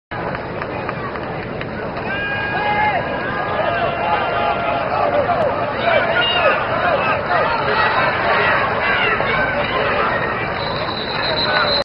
GZ: Charles 18-yd run
GZ: Charles 18-yd run - High School Football: McKinley vs. Gonzaga (8/28/2010)